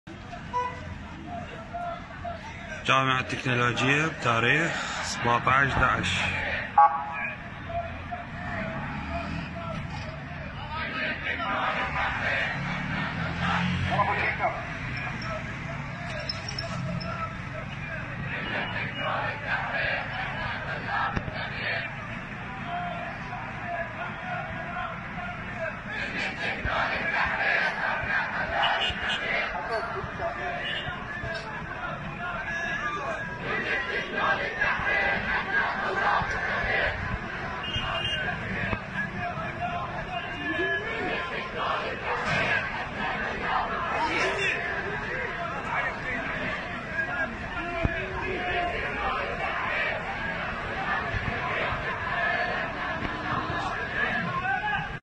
أنت_ترى | طلاب الجامعة التكنولوجية في بغداد يعلنون إضرابهم عن الدوام ومساندة متظاهري
ــــــــــــــــــــــ
يمكنكم إرسال ما توثقونه على تليغرام وواتساب